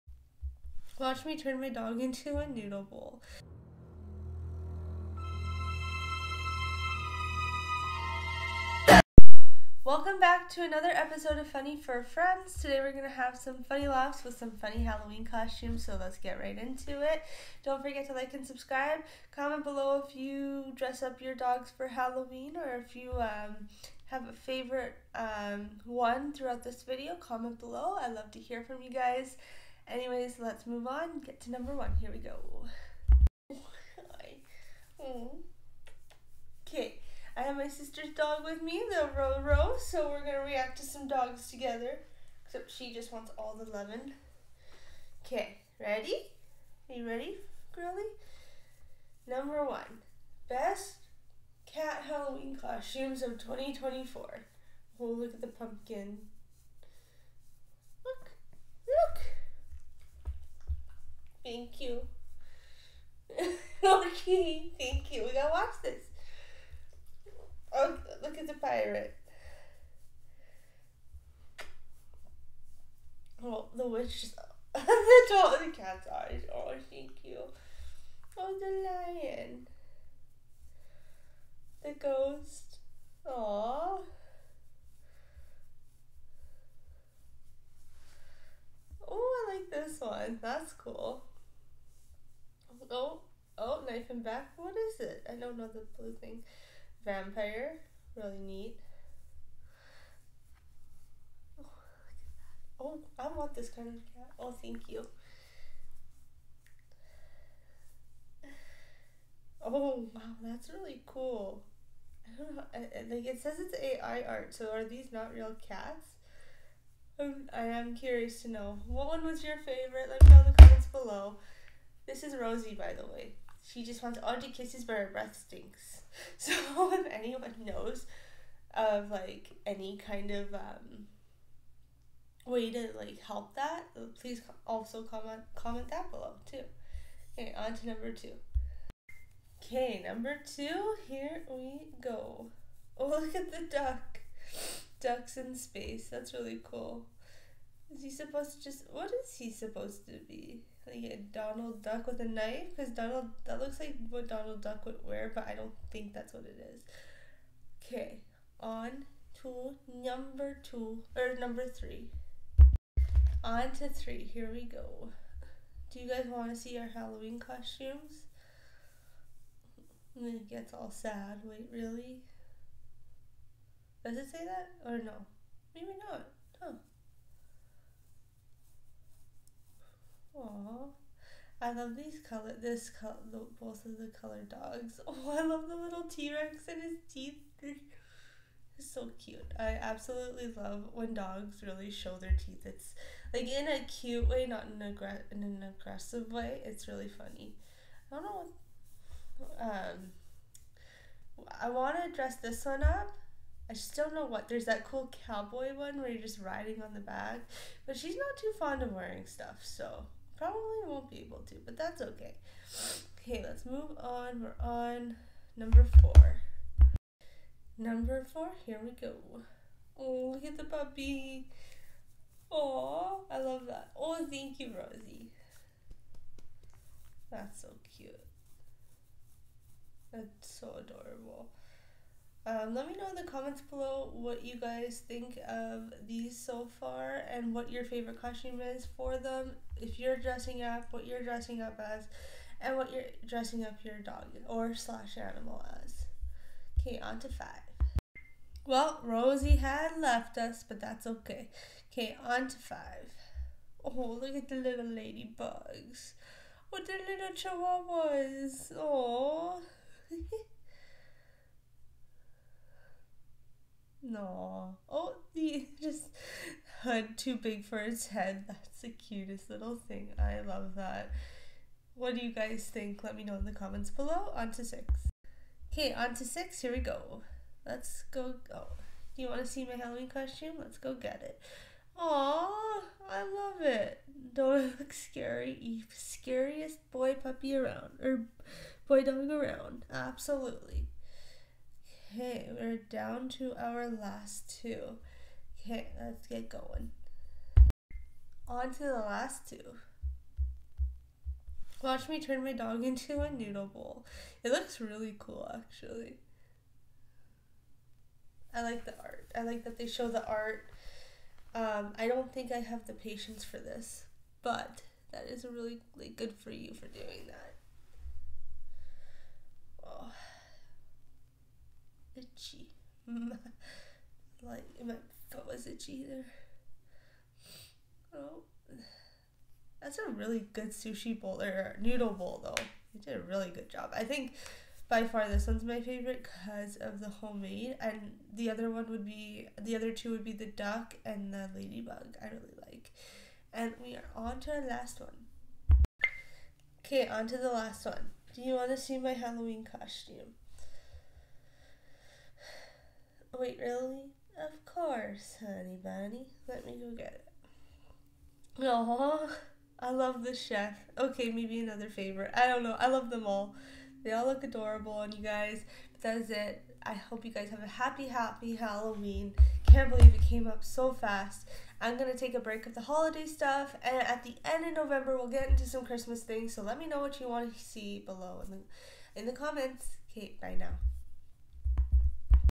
Spooky Cute! Funny Animals Dressed Up For Halloween
Get ready for a spook-tacular treat! Watch as these adorable animals dress up in their Halloween best, bringing a whole new level of cute to the spookiest of seasons. From puppies in pumpkin costumes to kittens in witch hats, these funny animals will have you howling with laughter. So sit back, relax, and enjoy this purr-fectly spooky compilation of Halloween cuteness.